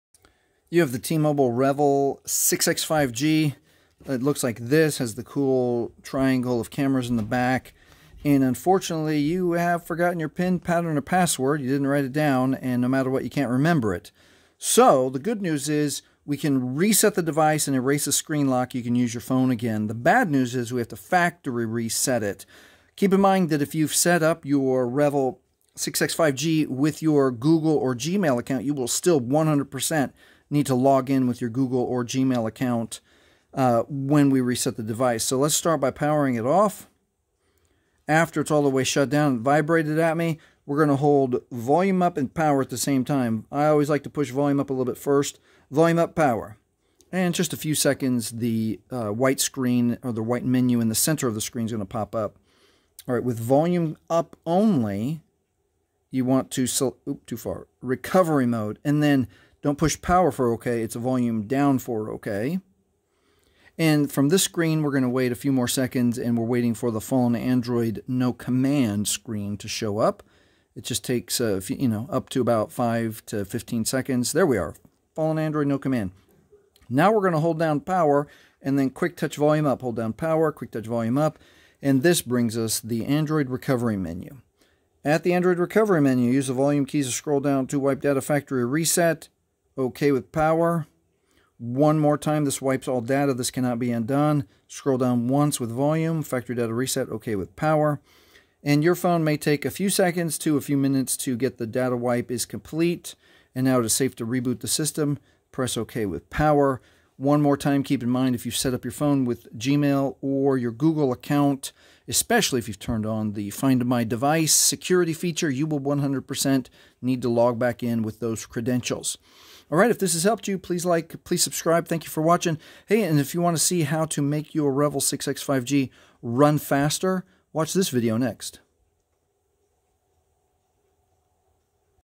Revvl 6X 5G - I Forgot my Pin Pattern or Password - How to Reset your TMobile REVVL 6X 5G
In this video, we'll show you how to factory reset your REVVL 6X 5G

(You Will ERASE ALL DATA FROM YOUR PHONE WITH THIS PROCESS)

Steps to factory reset REVVL 6X 5G:

HOLD VOL UP  + POWER - hold until menu shows on screen
Use Volume Up to Select RECOVERY MODE
HOLD POWER + QUICK TOUCH Volume Up to bring up Android Recovery Menu
Select WIPE DATA / FACTORY RESET
CONFIRM
VERIFY